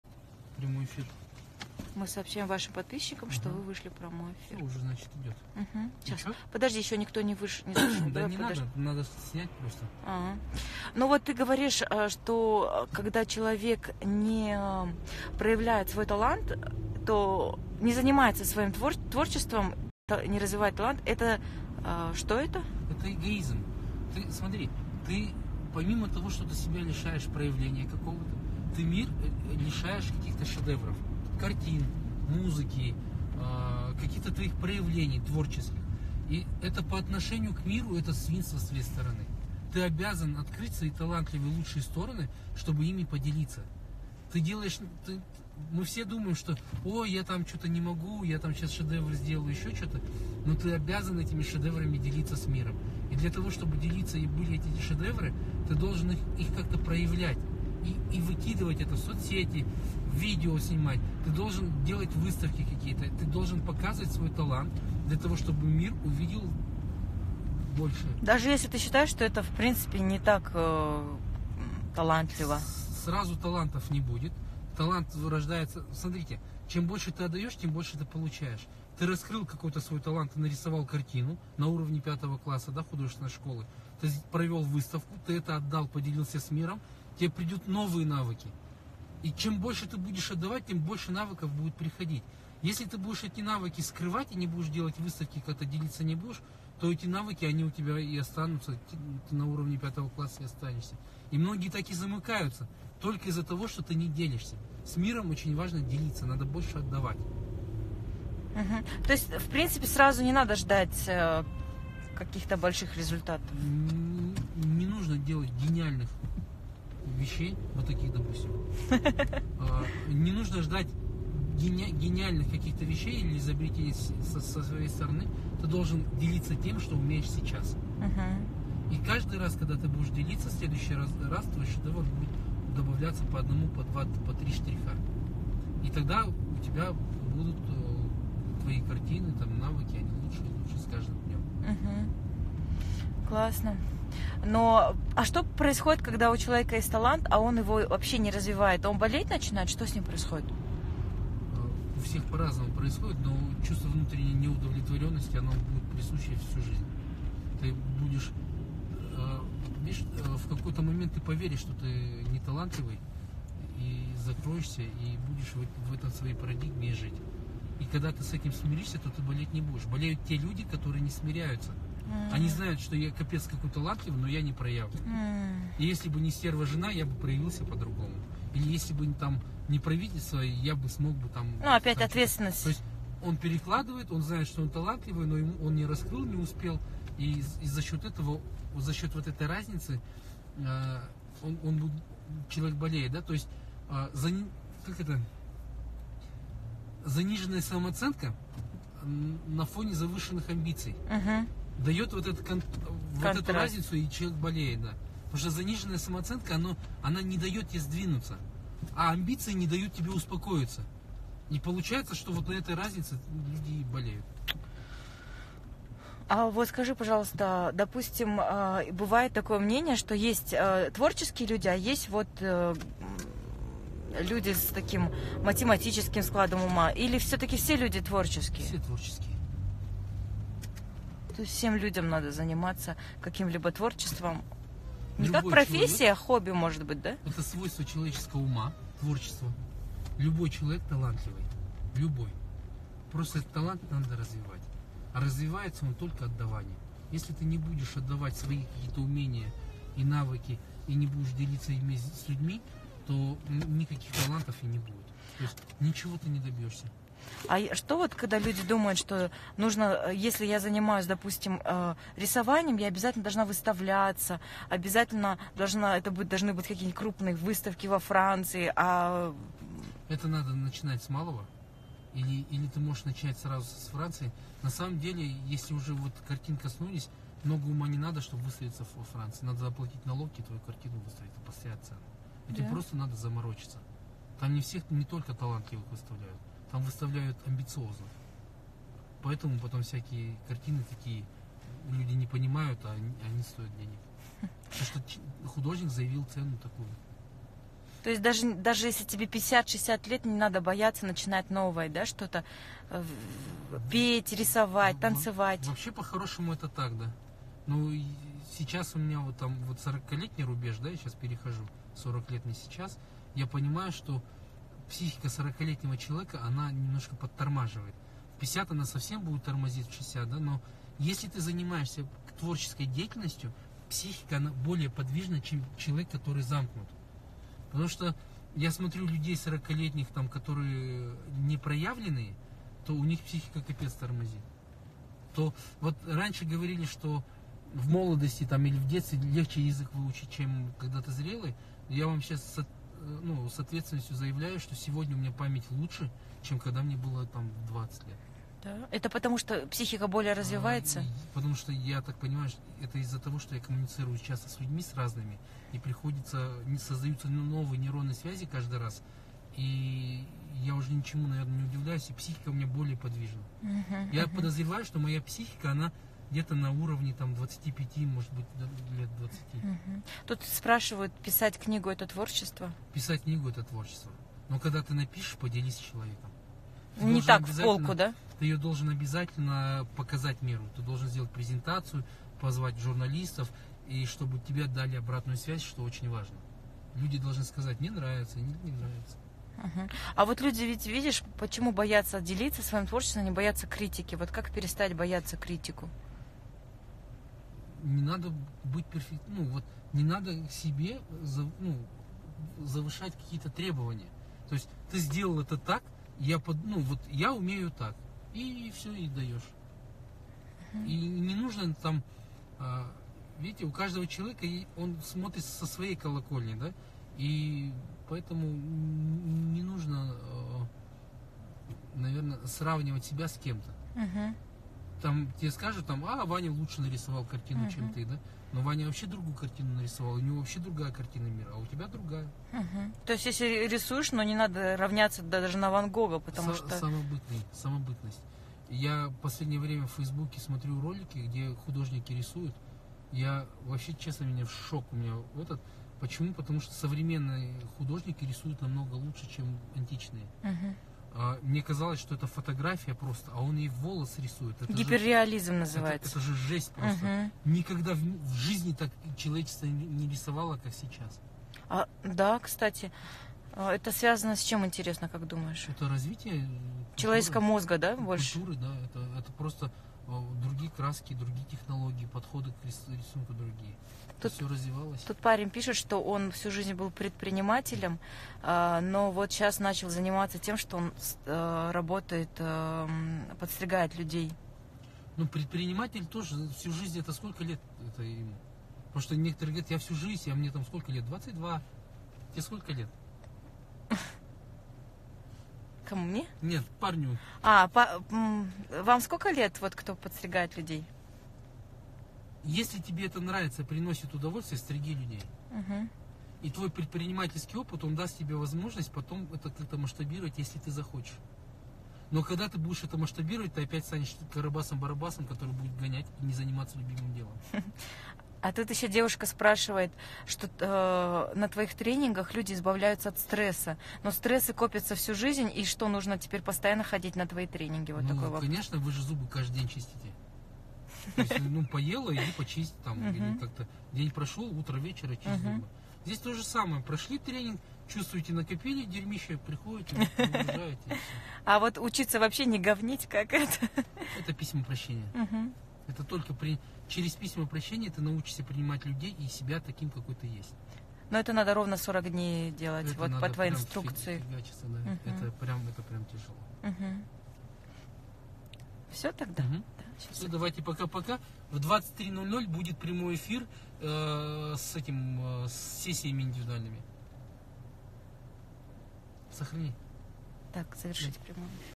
Раскрой свой потенциал. Талантлив ли я? Инстаграм прямой эфир Делись с миром своими талантами
Прямой эфир инстаграм. Спонтанная съемка.

Эмоциональная свобода
Контакты:
Перископ: @mastertor777